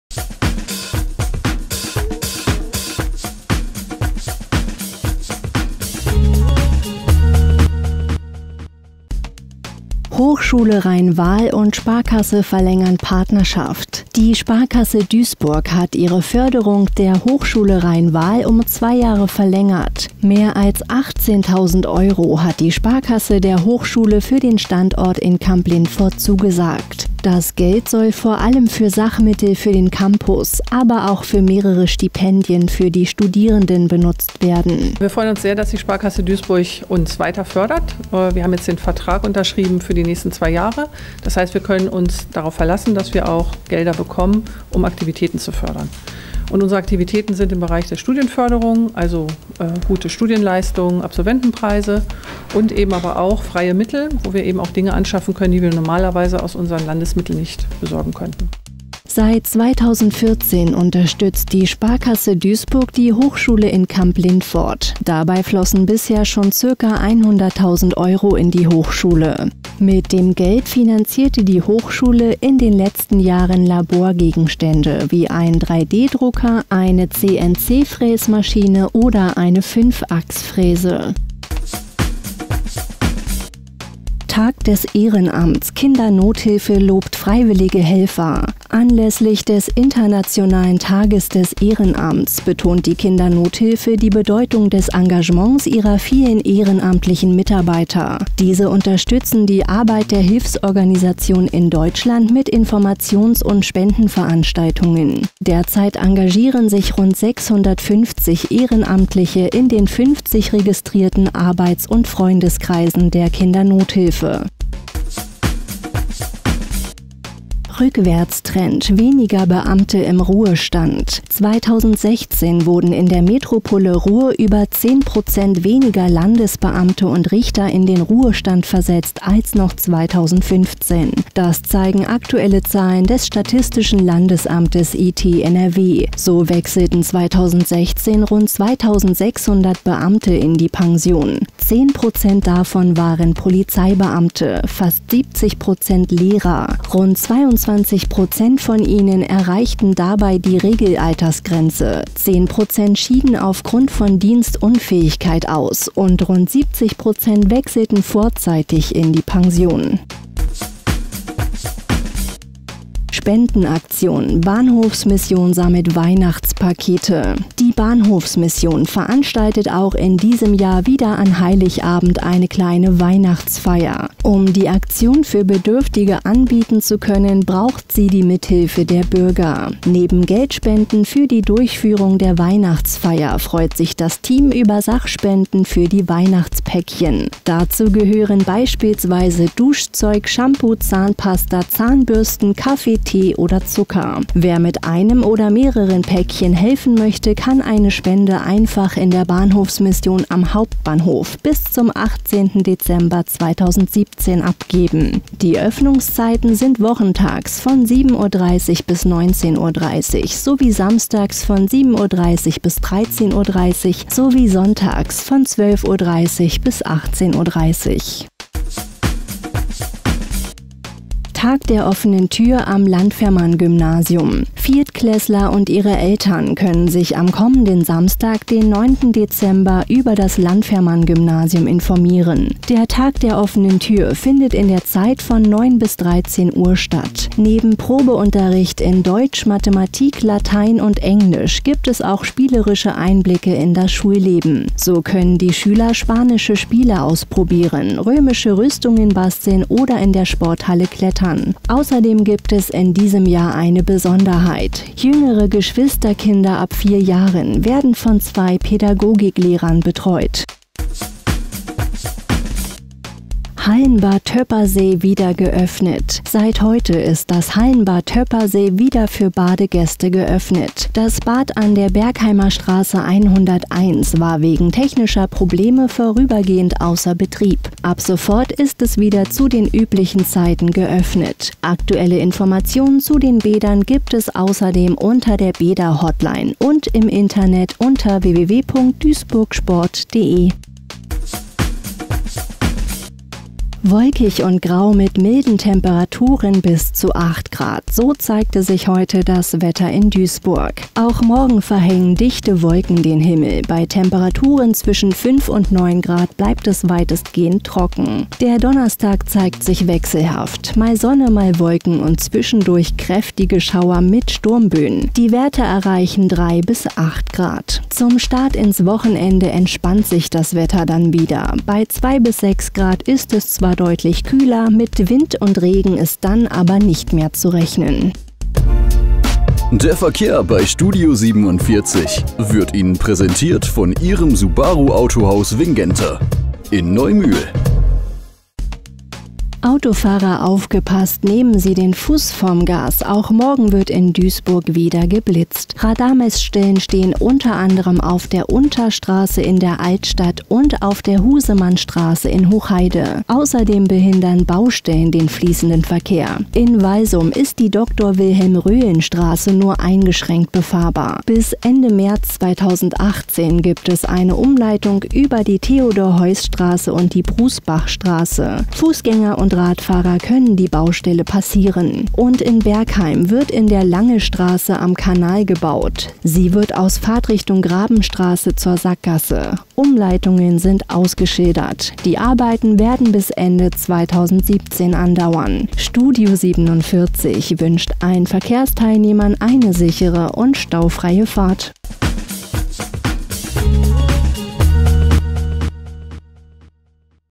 STUDIO 47 .nachrichten | 05.12.2017 | BILDUNGS-FINANZIERUNG FÜR WEITERE 2 JAHRE
Tag des Ehrenamts am 5. Dezember; Kindernothilfe lobt Engagement der Ehrenamtlichen | Aktuelle Daten des Landesamtes IT.NRW; 2.600 Beamte gingen 2016 in den Ruhestand | Geld- & Sachspenden gesucht; Helfer melden sich bis 18.12. in der Bahnhofsmission | Probeunterricht beim Tag der offenen Tür; Landfermann-Gymnasium: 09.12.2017 von 9:00 bis 13:00 Uhr | Technische Probleme behoben; Hallenbad Toeppersee wieder in Betrieb | Wetter | Verkehr